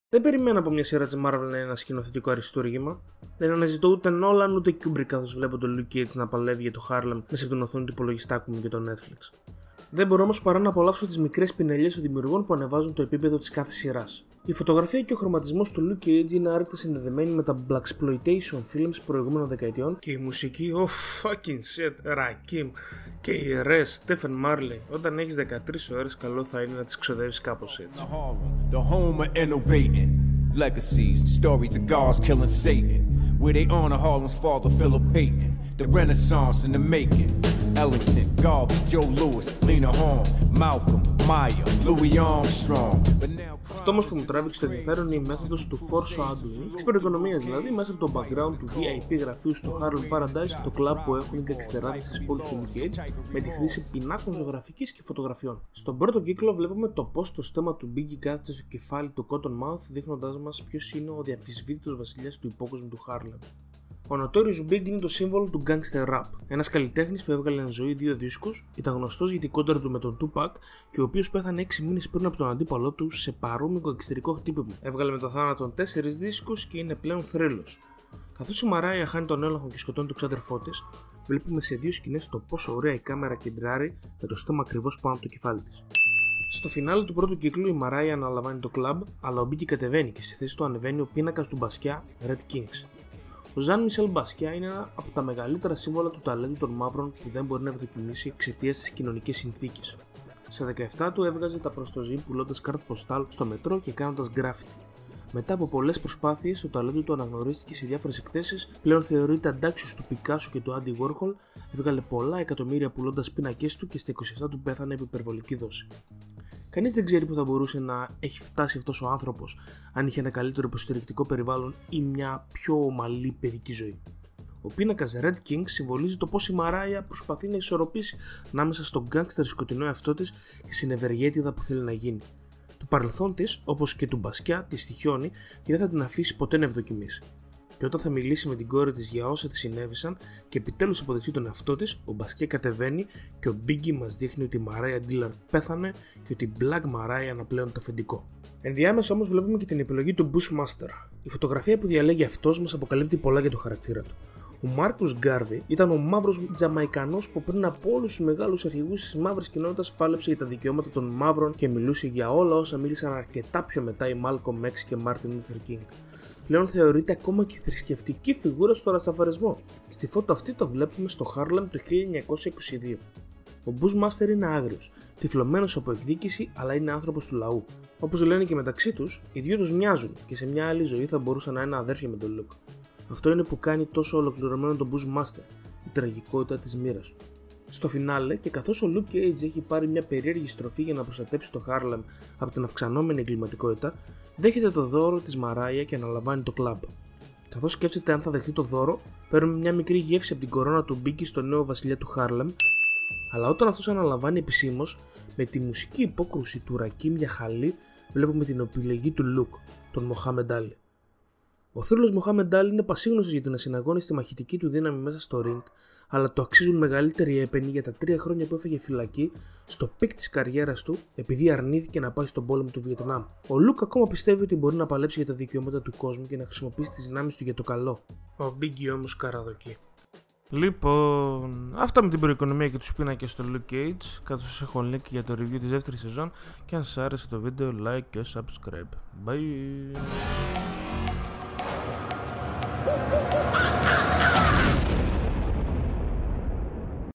Προ-οικονομία και Luke Cage | Monkey Thoughts
H χρήση φωτογραφιών και πινάκων ζωγραφικής ως μέσω προ-οικονομίας στο Luke Cage, είναι η σκηνοθετική μαγκιά του δημιουργικού team που μου τράβηξε το ενδιαφέρον στη πρώτη season αλλά πολύ περισσότερο στην δεύτερη.
Ακολουθούν Spoilers

Enjoy και περιμένω τα σχόλια σας